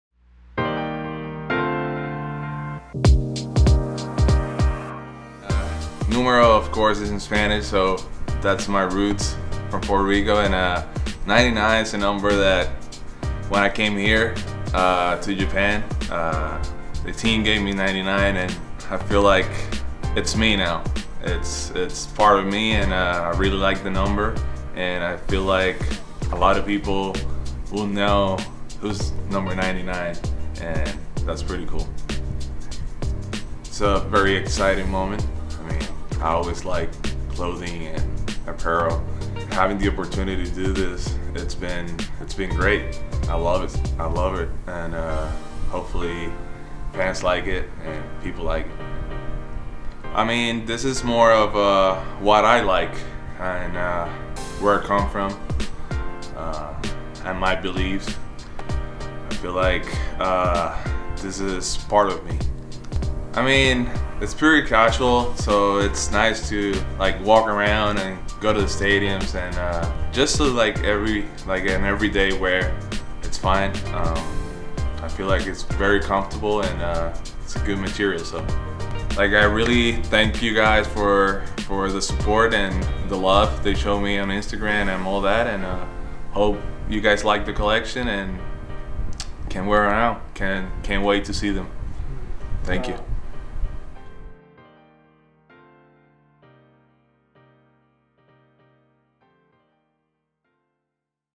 Numero99 by Neftali Soto Special Interview
「Numero99 by Neftali Soto」
横浜DeNAベイスターズ所属のネフタリ・ソト選手が手掛ける
オリジナルアパレルコレクション。
ソト選手が企画からデザイン、生地選びすべてに携わり、
支えてくださっている方々への想いや、ソト選手の大切なルーツをアイテムにこめました。

Original clothing collection produced by Neftali Soto from the Yokohama DeNA Baystars.
Soto has joined the project from planning, design deciding to material selecting.
Please check the items that Soto has put his careful thoughts and roots for the people who support and encourage him.